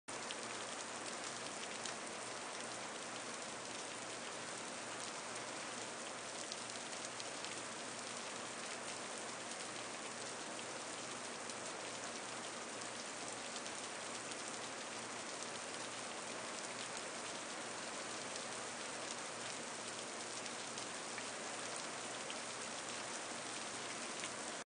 Eastern box turtle in the rain, July 22, 2013, Ocean Springs, MS
They become active when it rains and fill up on earthworms that come to the surface when the ground gets saturated.